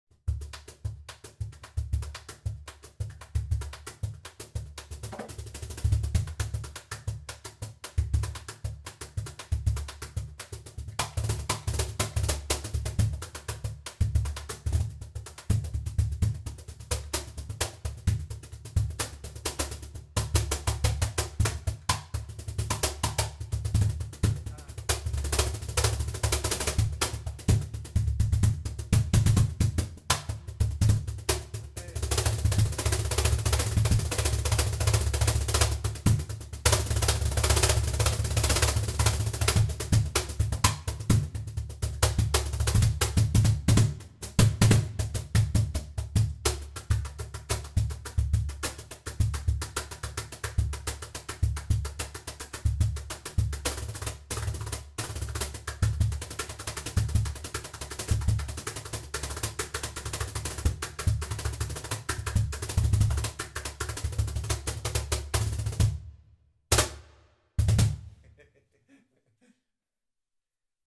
Israel Suarez Piraña with cajon DG Tokayo
Checking a Tokayo model. Piraña is not a DG cajon endorser, but he is currently preparing a tuitional video produced by DG De Gregorio.